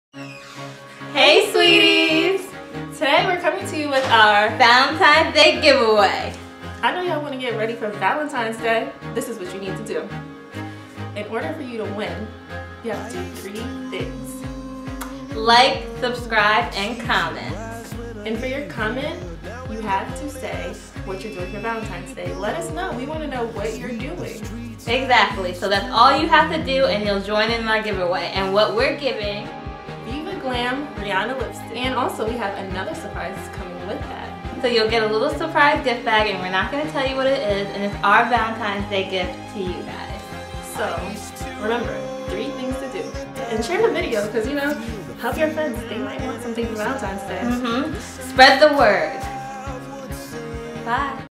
Valentines Day Giveaway!!!
We are giving away One Viva La Glam Rihanna Lip Stick & a little surprise gift to one of our luck subscribers!

There are 3 Simple Rules to follow in order to be entered to win:
2. Like
3. Comment with Valentines Day Plans

Last Chance to enter February 13th 2014

Winner will be announced February 14th, 2014

Enjoy Sweeties

(all sweeties will remain anonymous)